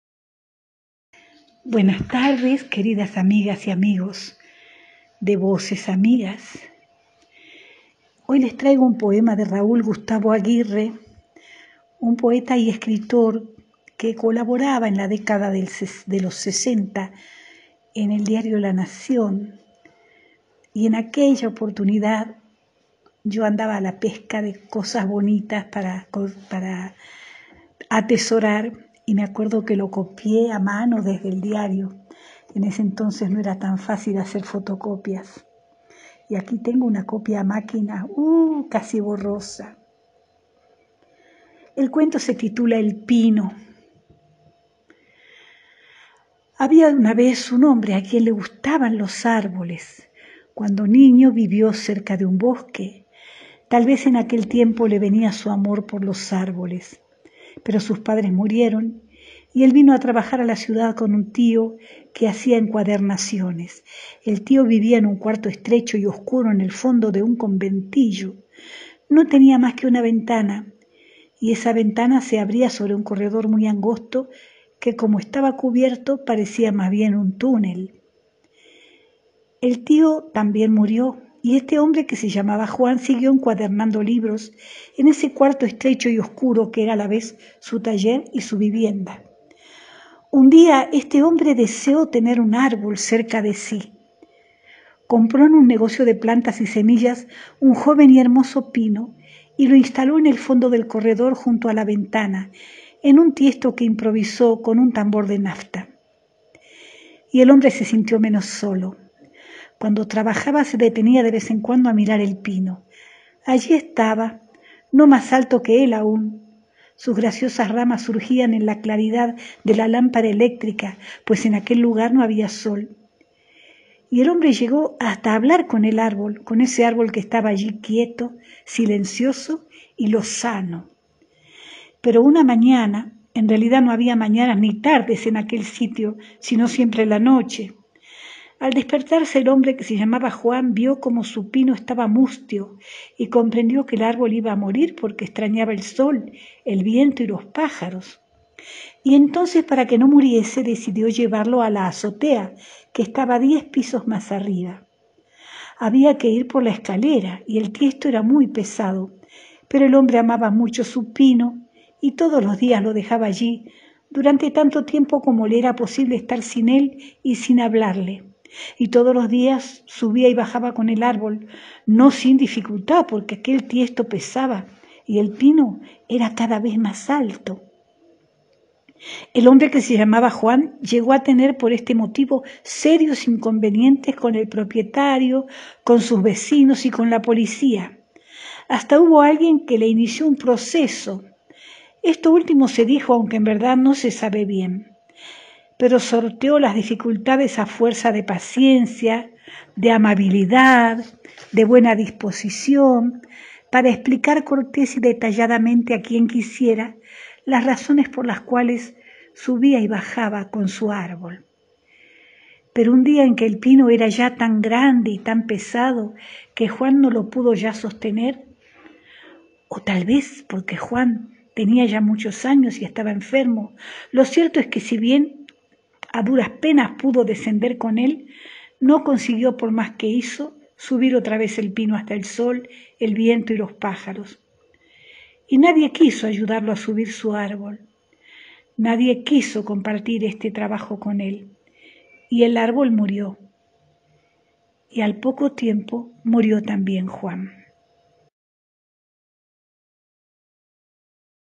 Ana María nos lee "El pino", de Raúl Gustavo Aguirre.
Raúl Gustavo Aguirre (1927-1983) fue un poeta argentino. Adscrito al grupo del invencionismo, mostró una cierta influencia surrealista.